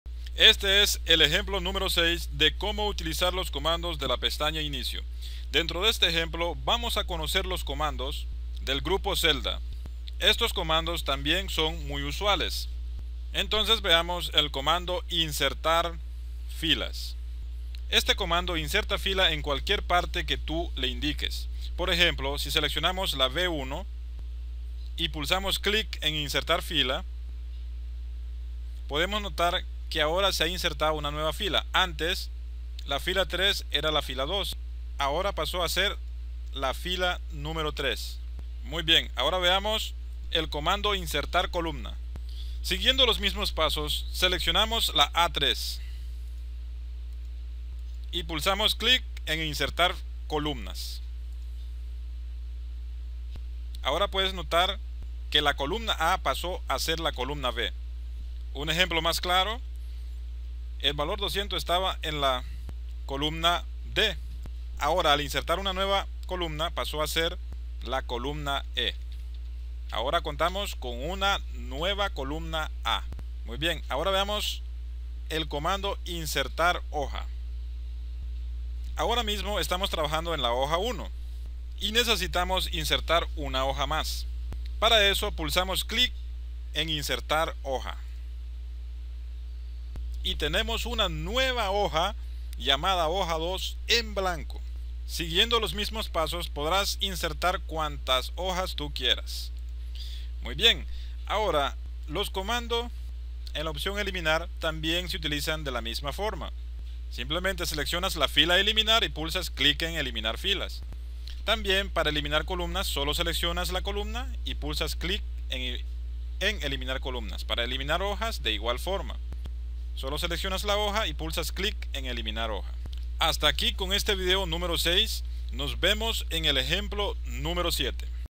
Ejemplo Nº 6 - Como utilizar los Comandos de la Pestaña Inicio
Ejemplo 6 de como utilizar los comandos de la Pestaña Inicio de Excel Online.. Haz clic en el enlace, para conocer mas de excel online: